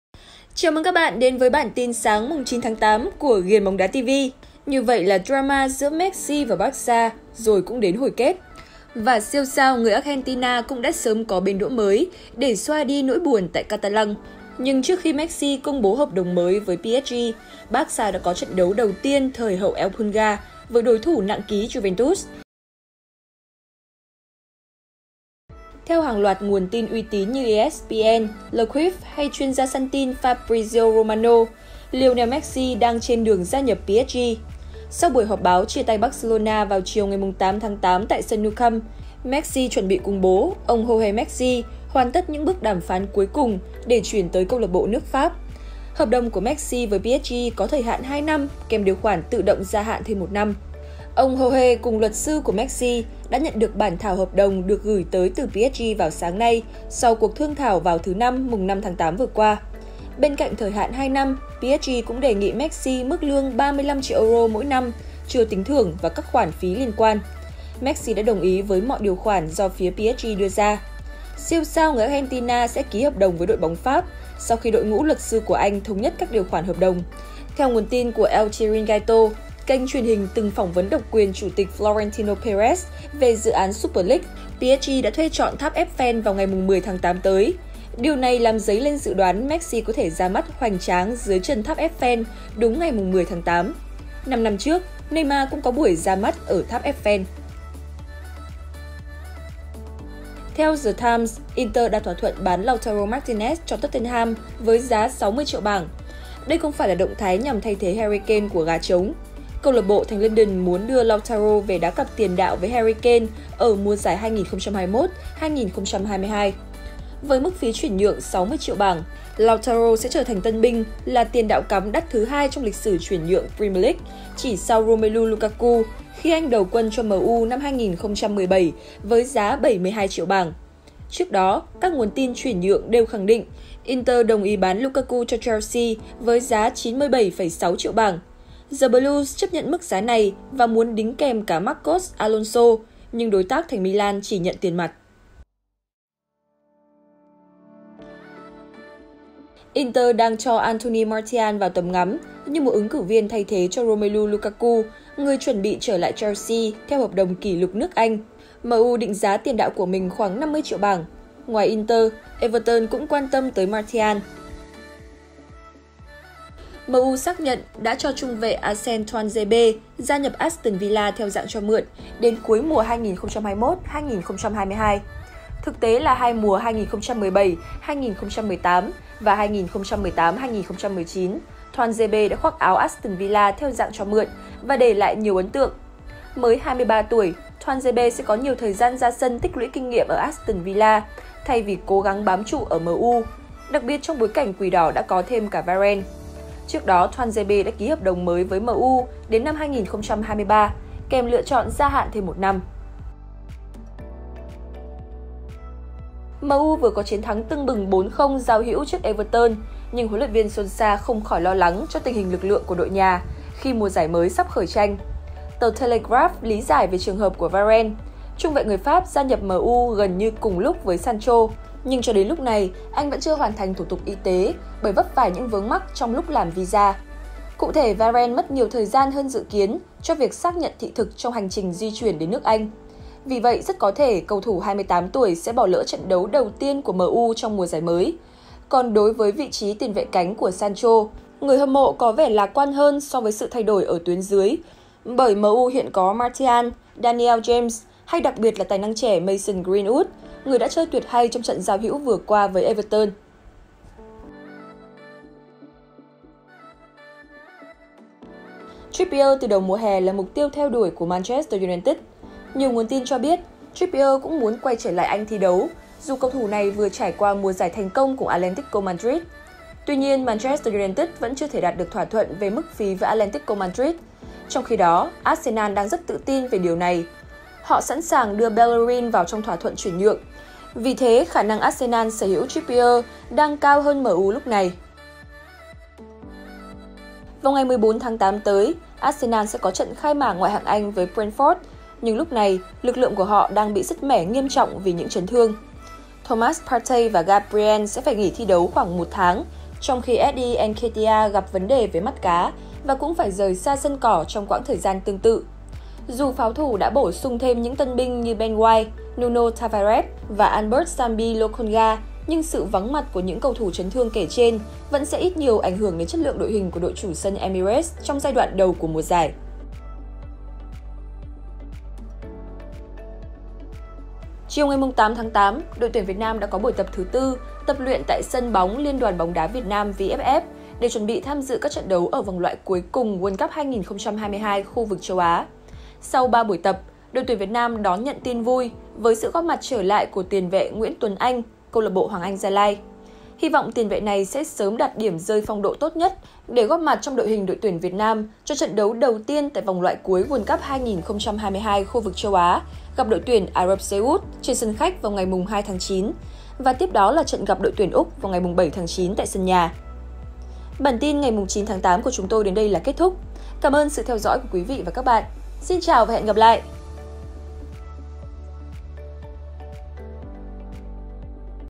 TIN BÓNG ĐÁ SÁNG 9/8 | Messi ĐÃ ĐỒNG Ý đến PSG, Ronaldo thua Barcelona ngay sau Messi ra đi!
Theo Fabrizio Romano - Messi gia nhập Paris Saint-Germain với hợp đồng hai năm 🤝🇫🇷
 Barcelona thắng 3-0 Juventus tại Cup giao hữu Joan Gamper trên sân Camp Nou